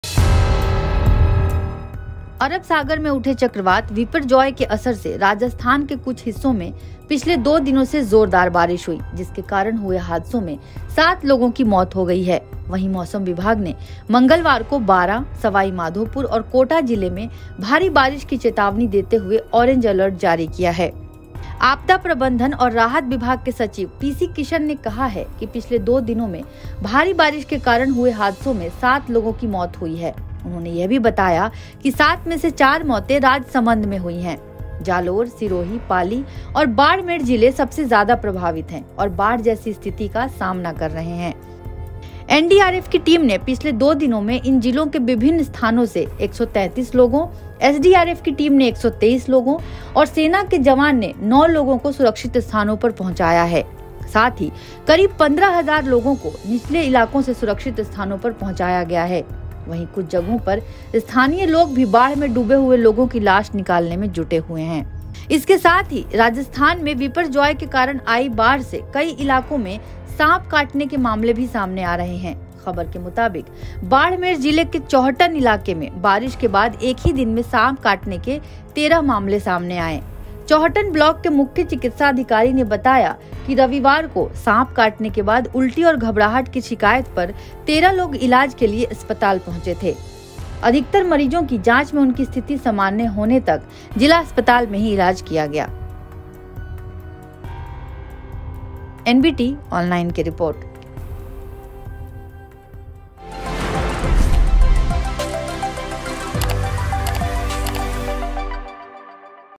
Biporjoy Cyclone के कारण Rajasthan में हुई 7 लोगों की Death, स्थानीय लोग भी निकाल रहे लाश | NBT
Biporjoy के कारण rajastha में कई जगहों पर हालात खतरनाक हो गए हैं. जालोर, सिरोही, पाली और बाड़मेर जिले सबसे ज्यादा प्रभावित हैं और बाढ़ जैसी स्थिति का सामना कर रहे हैं. आपदा प्रबंधन और राहत विभाग के सचिव पीसी किशन ने कहा कि पिछले दो दिनों में बारिश के कारण हुए हादसों में सात लोगों की मौत हुई है. उन्होंने यह भी बताया कि सात में से चार मौतें राजसमंद में हुईं. इन हादसों के बाद लाशों के निकालने काम शुरू हो गया है. वहीं, कुछ जगहों पर स्थानीय लोग भी बाढ़ में डूबे हुए लोगों की लाश निकालने में जुटे हुए हैं.